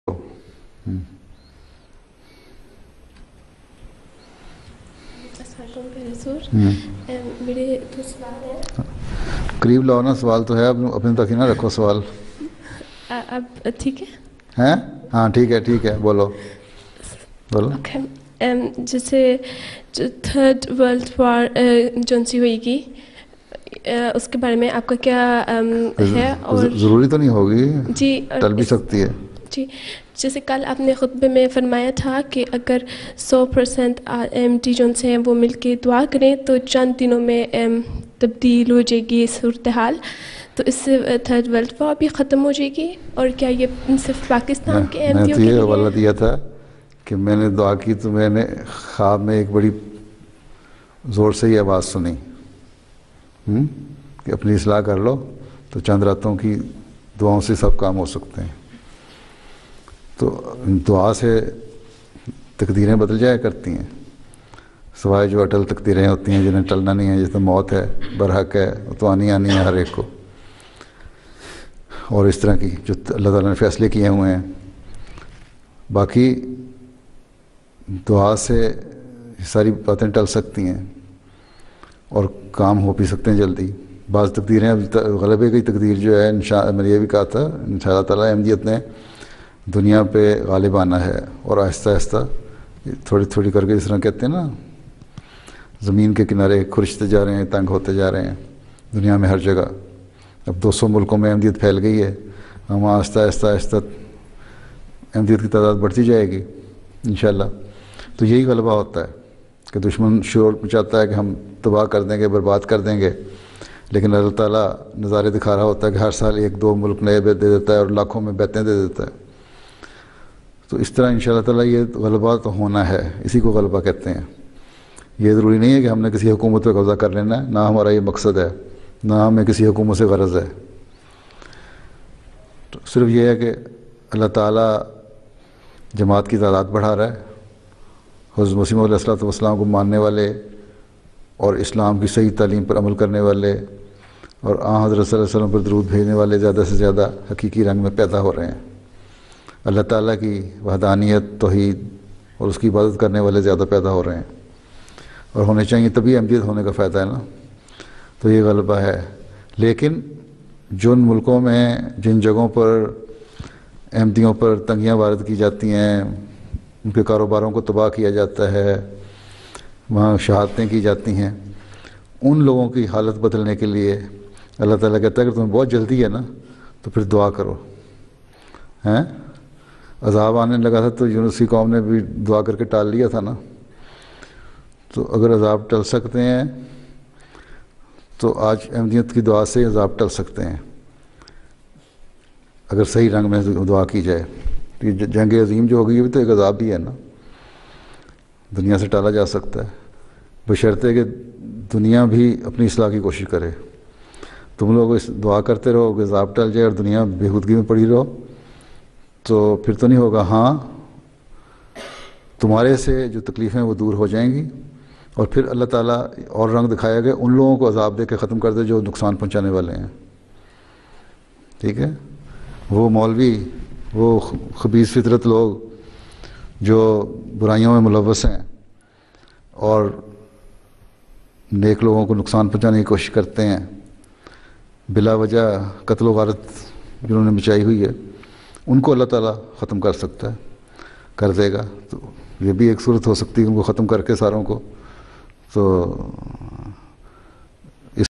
Prayers of Ahamdis and 3rd World War...
Gulshan Waqf Nau, Lajna and Nasirat, Germany October 8, 2011
A Question about 3rd World War:  As Huzoor stated that If all Ahmadis pray together, is it possible that danger of 3rd World War be deferred?
ایک واقفہ کا سوال کہ حضورِ انور نے فرمایا تھا کہ اگر تمام احمدی مل کر دعائیں کریں تو تیسری جنگِ عظیم کا خطرہ ٹل سکتا ہے، کیا ایسا ممکن ہے؟